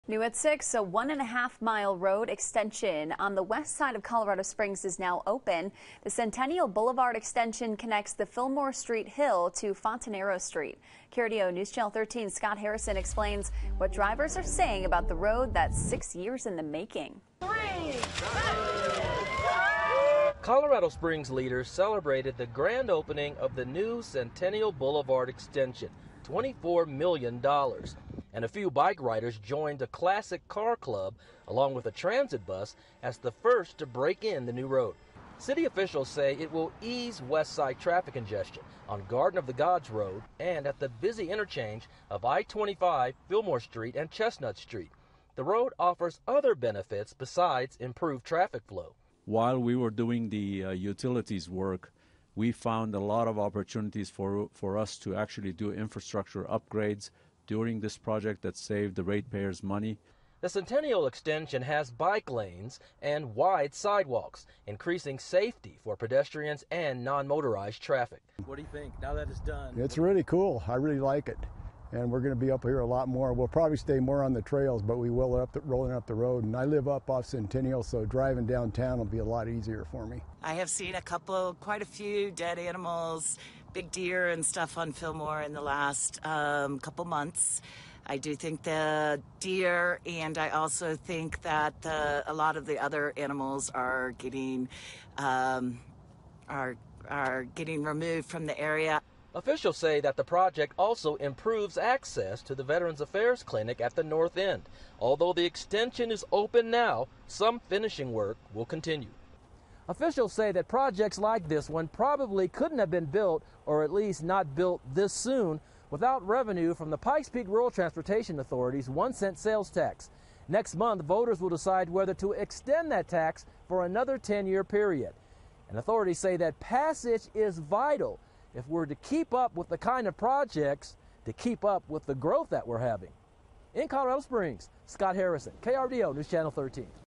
Extension of Centennial Boulevard in west Colorado Springs completed
The project took six years and cost $24 million.